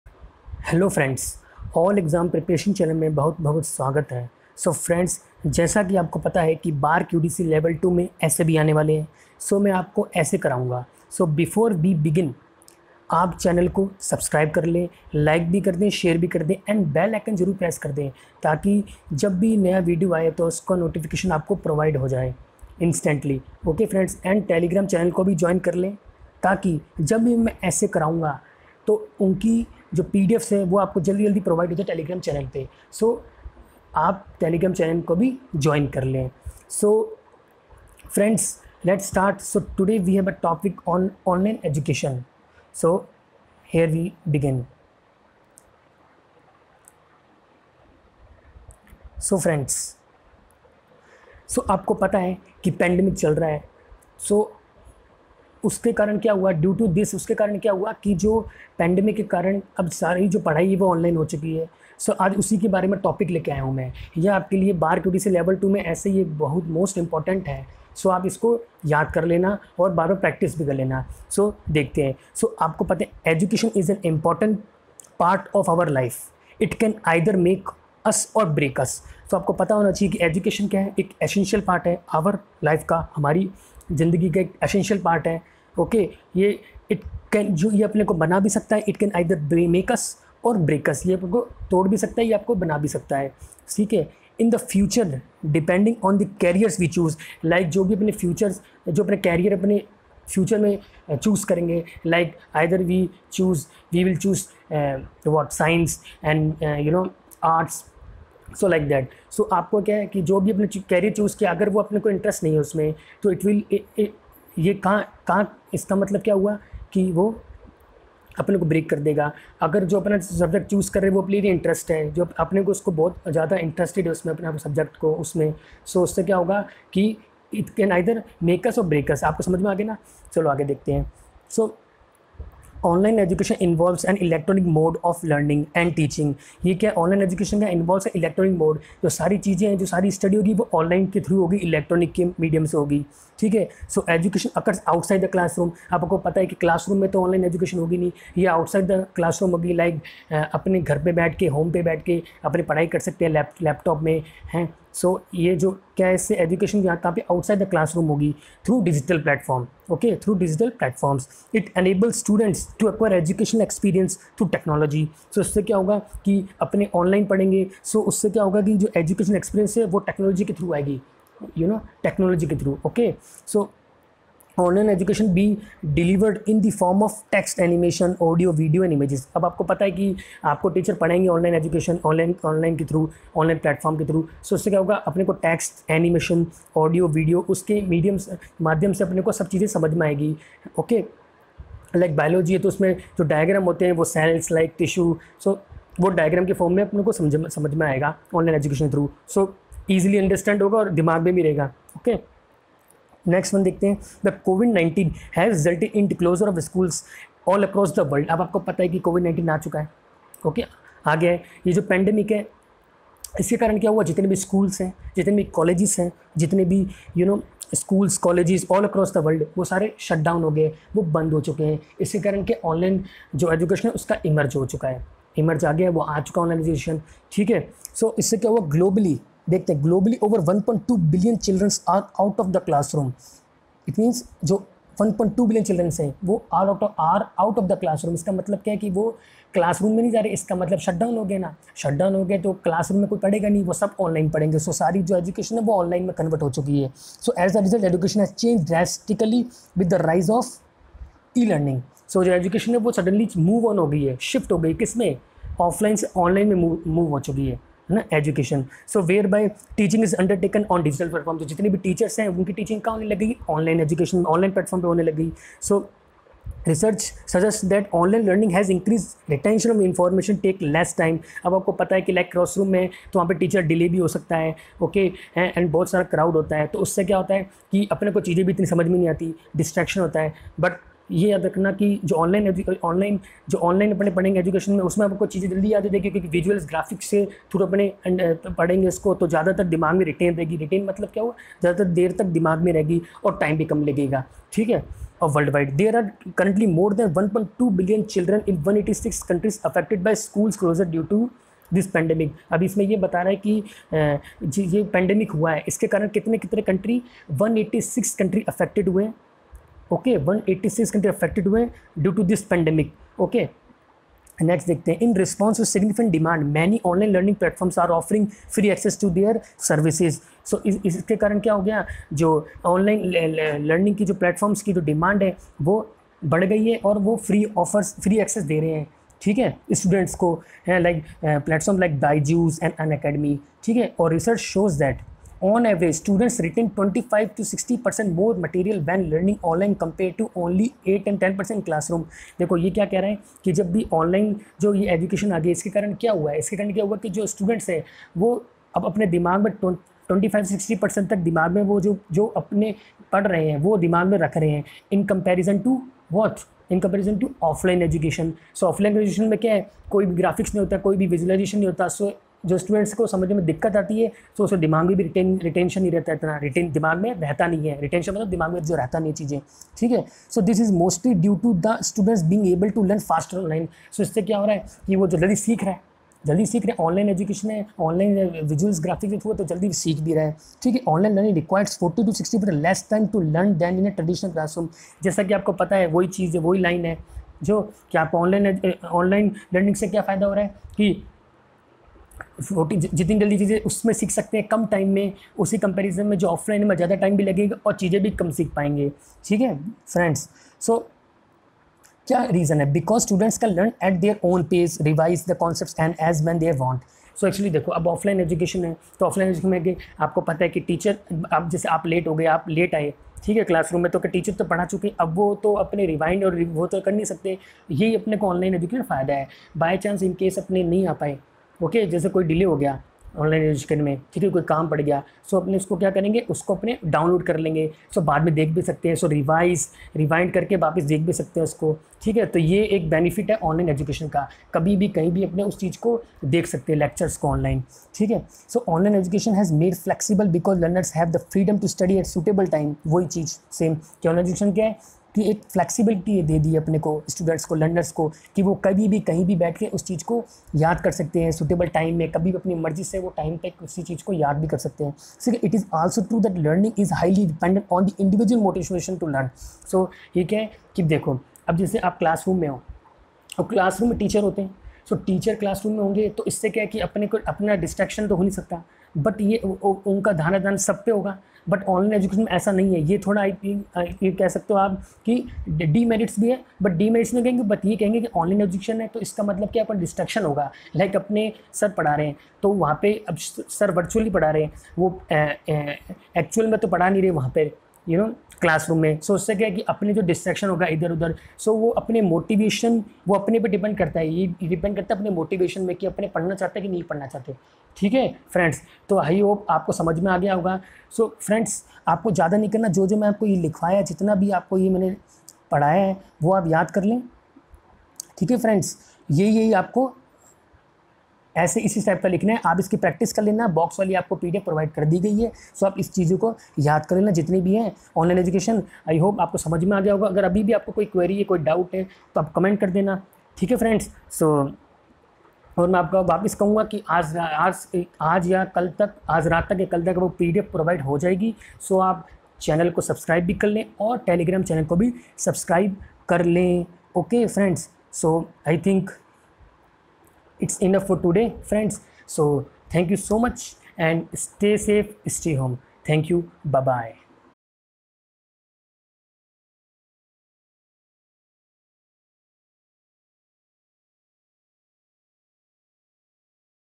BARC UDC Level-2 | 💻 Online 📚 Education | Essay | Descriptive Paper
Hello Students

Yeh "BARC UDC Level 2 Descriptive Essay topic on Online Education" ki video hai.

Video acchi lge toh like kre, share kriye jisse ki bakki ko bhi madat milti rhe aur vo bhi exam mein accha kre.